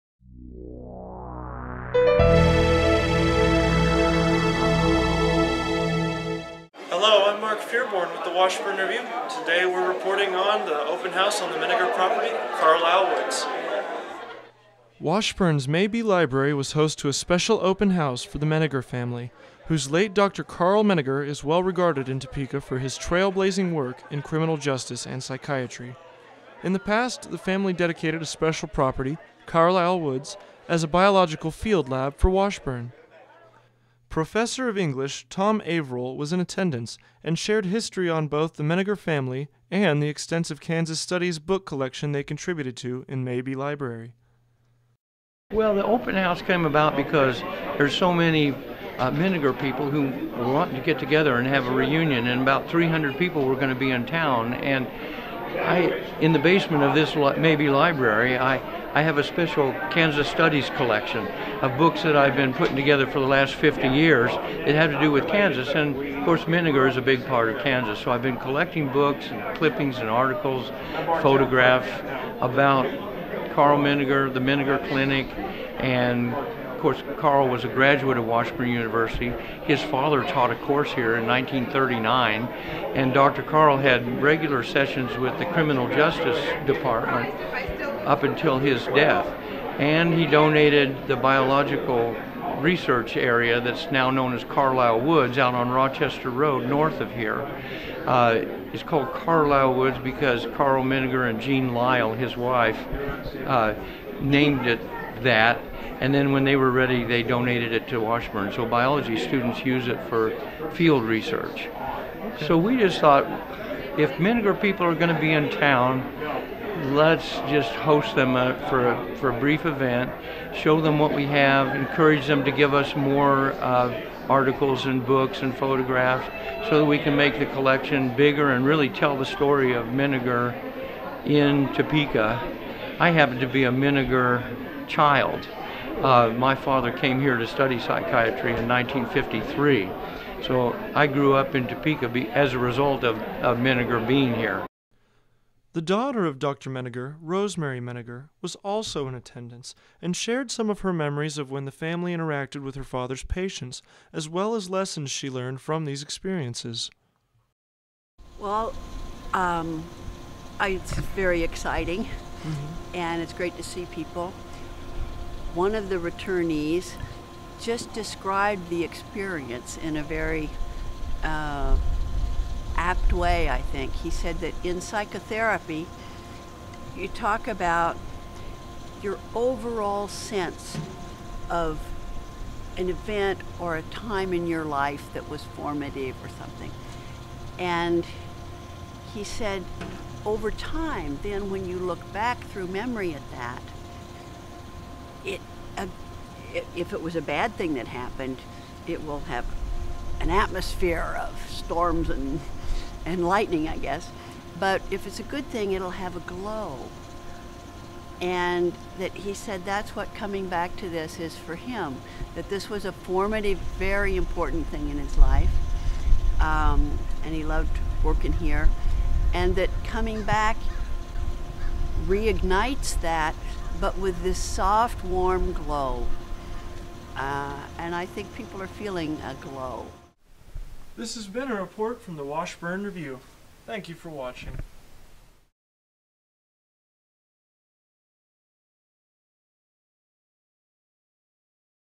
Mabee Library hosts open house for Menninger family
Reporter Mark Feuerborn visits the open house for the Menninger family, whose Dr. Karl Menninger was a pioneer in psychiatry and donated the Karlyle Woods property to the university for a biological field lab.

Director:  Mark Feuerborn
Producer: Annalee Lubeski and Mark Feuerborn
Videographer: Annalee Lubeski
Anchor: Mark Feuerborn